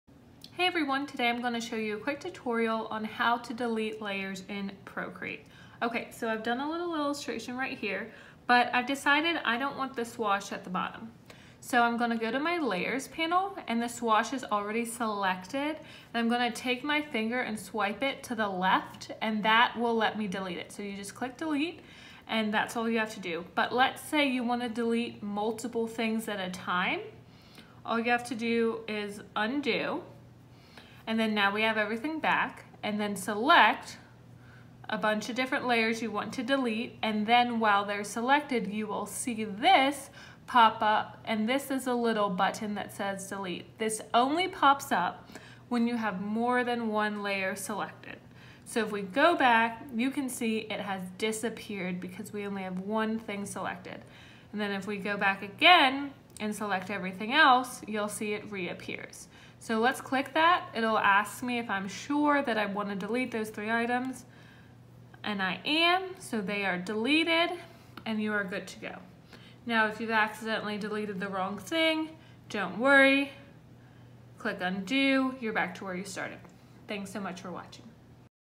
How To Delete Layers In Procreate
Knowing how to delete layers in Procreate can super helpful when creating designs. Deleting unused layers can help you organize your project better!

SHOP MY DIGITAL PRODUCTS:

WHAT I'M USING:

12.9 iPad Pro (3rd Gen)
Apple Pencil (2nd Gen)